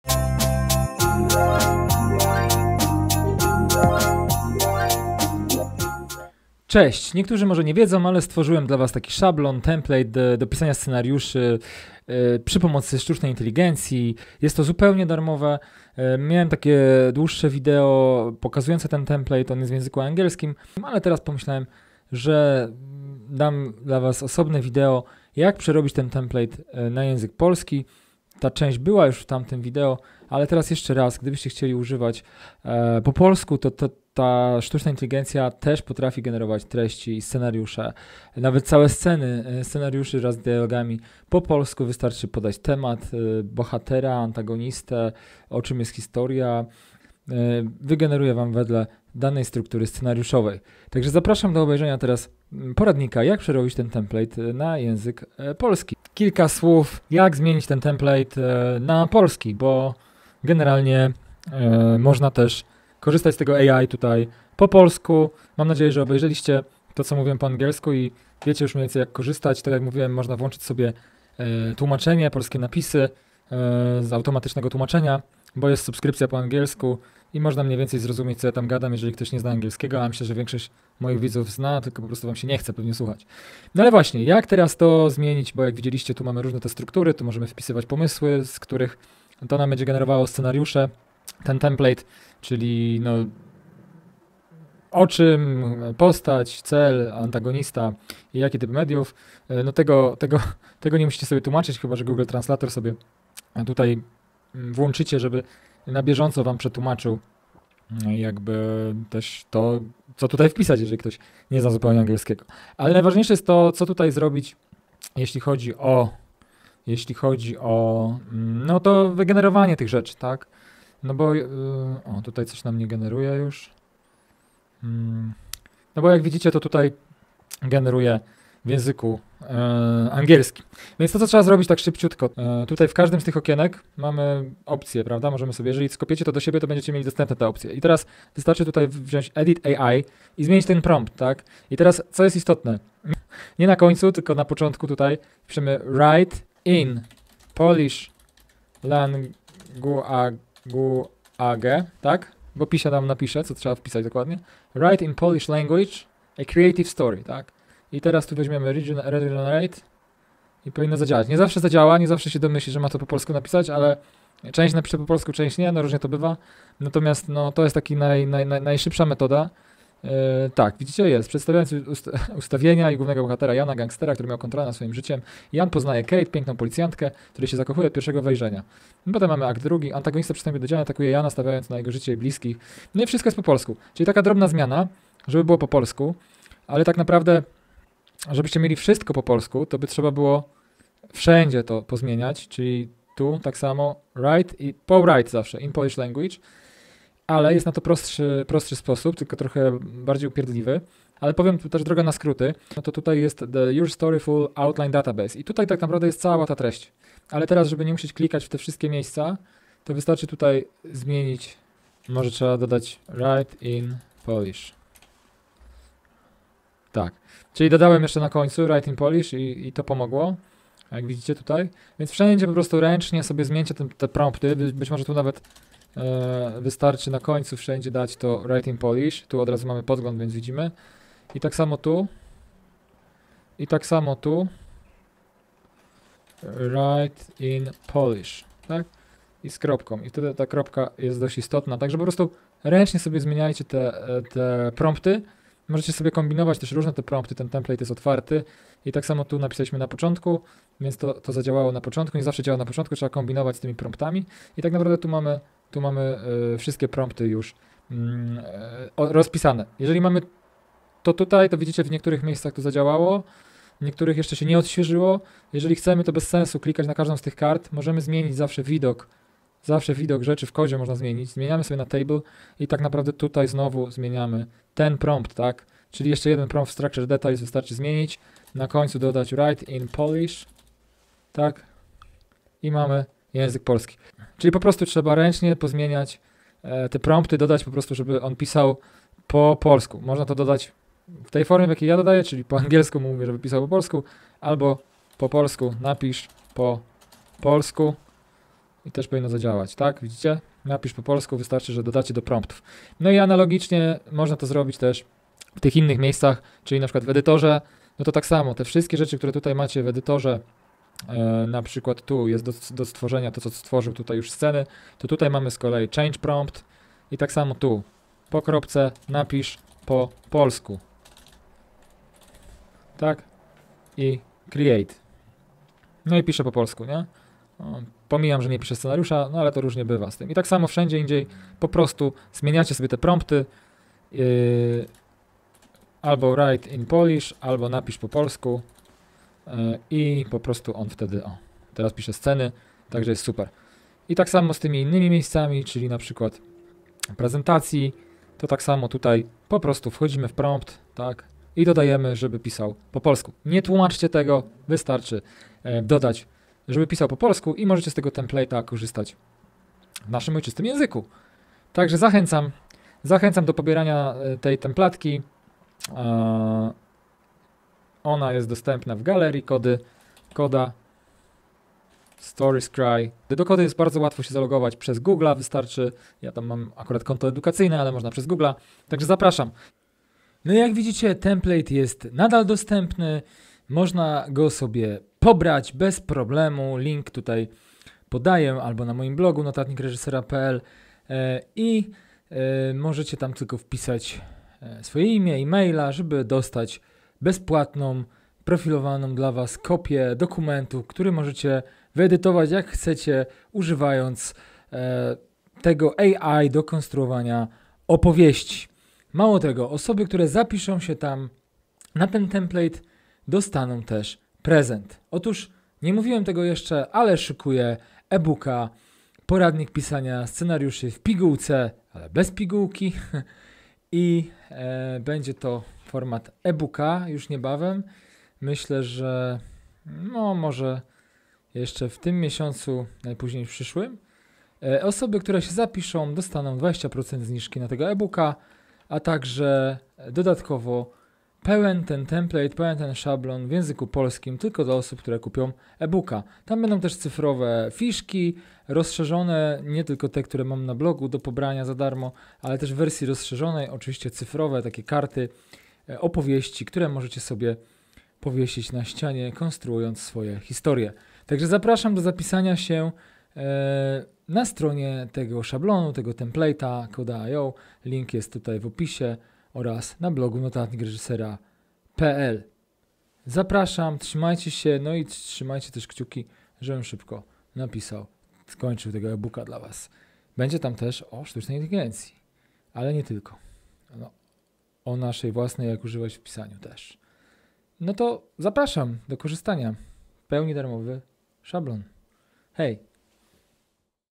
Pisanie opowieści z Coda AI po polsku! Jak przerobić mój szablon na PL
🎬 W tym vlogu na YouTube pokażę Ci, jak przerobić ten mój darmowy szablon struktury opowieści na język polski! Szablon ten jest niezbędnym narzędziem dla pisarzy i twórców, którzy chcą tworzyć przekonujące narracje. Skupię się na dwóch popularnych ramach, Trzy-Aktowej Strukturze i Podróży Bohatera (Autora), i wyjaśnię, jak można je wykorzystać do tworzenia historii dla różnych mediów, takich jak książki, filmy, gry, kampanie marketingowe i produkty.

Ponadto pokażę Ci, jak dostosować szablon do języka polskiego i jak wykorzystać go do tworzenia opowieści w naszym ojczystym języku. Nauczysz się, jakie elementy struktury opowieści są kluczowe, jak je uzupełnić, aby stworzyć spójną i atrakcyjną opowieść.

Ten tutorial vlog jest niezbędny dla każdego, kto chce stworzyć przekonujące historie w języku polskim!